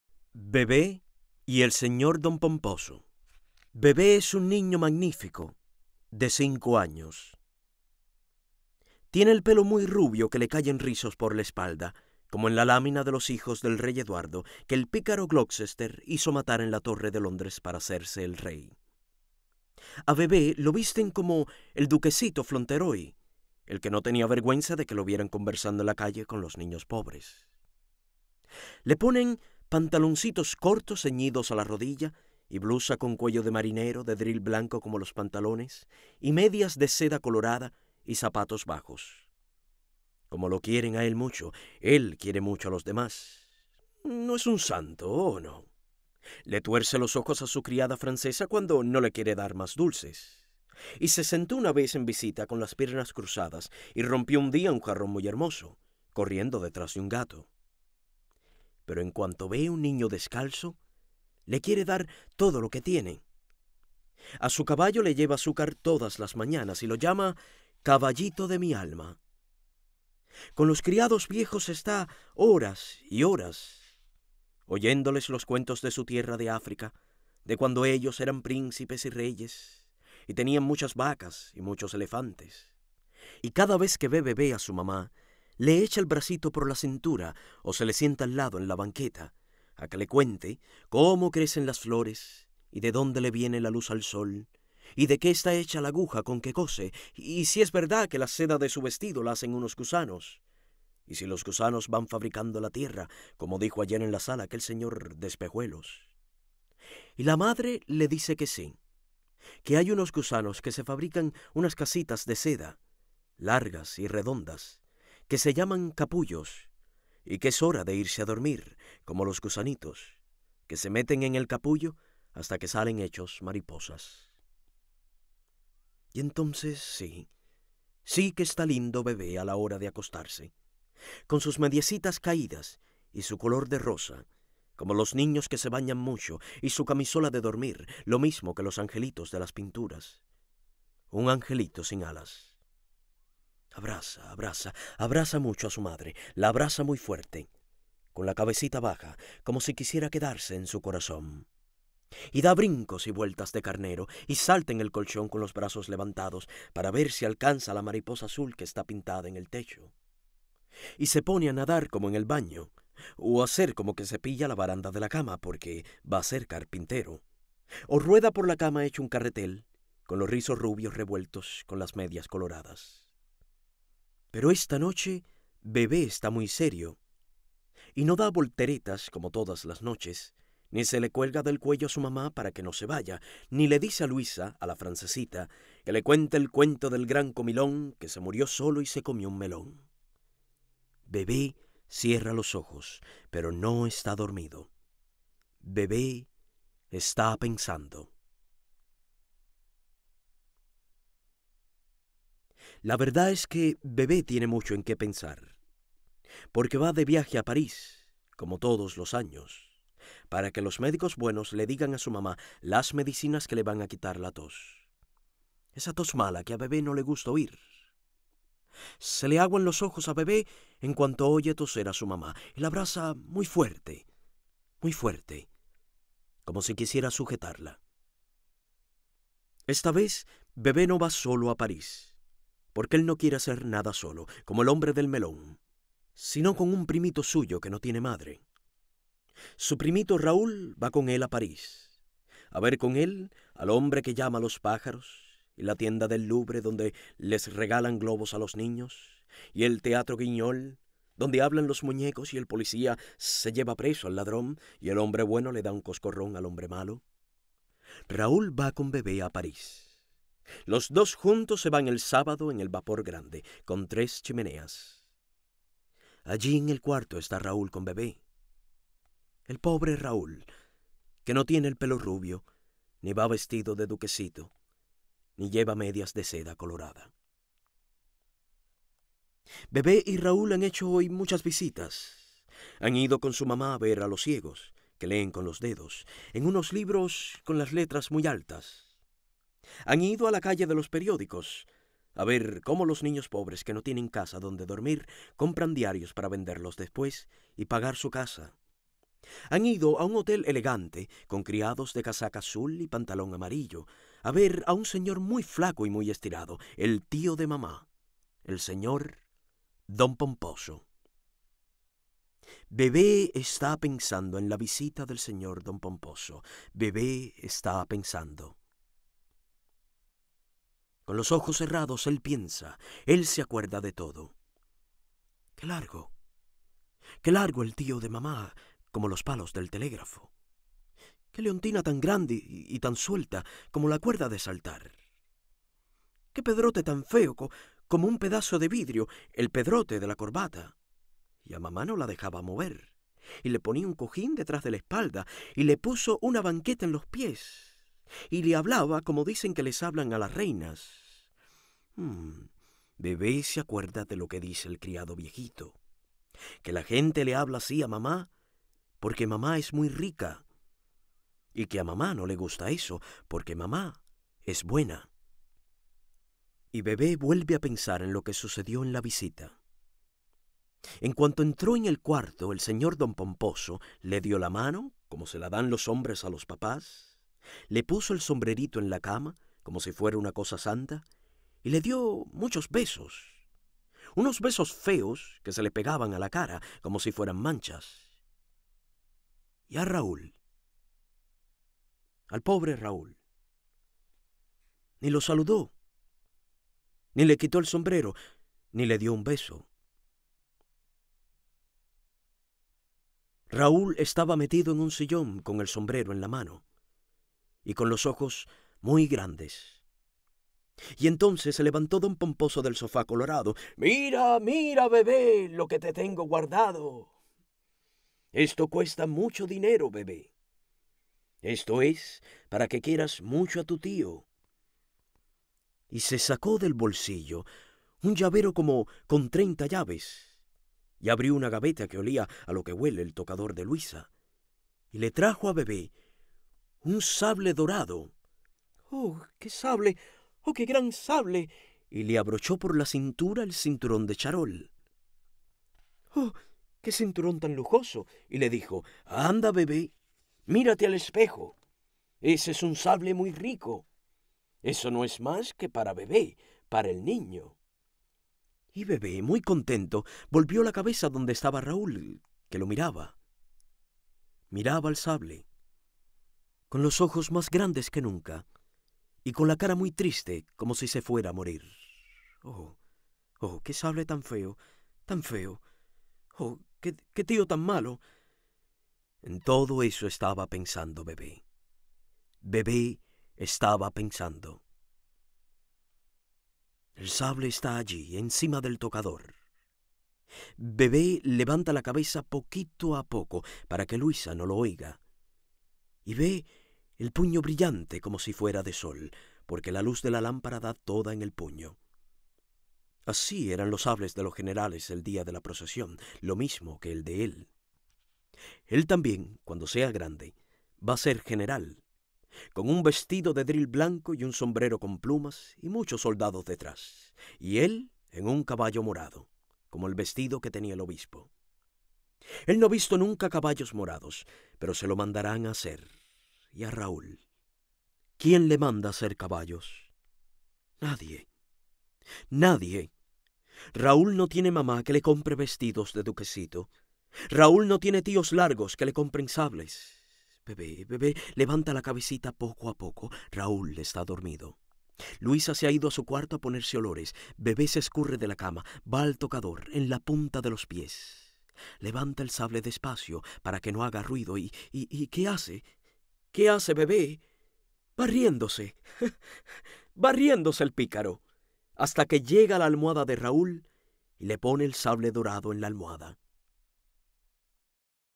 7-Bebé y el señor Don Pomposo
Lectura homenaje por el 130 aniversario de La Edad de Oro, revista mensual para niños escrita por José Martí en la ciudad de New York y publicada en Julio de 1889.
Cuentos, poesías, relatos y ensayos son los textos que aparecen en los cuatro números de la revista y muestran la proyección universal de los ideales martianos en la búsqueda de la justicia, el amor y el conocimiento.
“ha de ser para que ayude a lo que quisiera yo ayudar, que es a llenar nuestras tierras de hombres originales, criados para ser felices en la tierra en que viven, y vivir conforme a ella”.
Sobre el narrador: Yamil Cuéllar es un actor cubano que desde San Francisco, California realiza su programa Lecturas de Yamil Cuéllar.
Agradecimientos especiales a la escritora Belkys Pulido, al historiador cubano Ramón Guerra Díaz, al Doctor en Ciencias e investigador cubano-dominicano Alejandro Herrera Moreno y la fundación cultural Enrique Loynaz en Santo Domingo.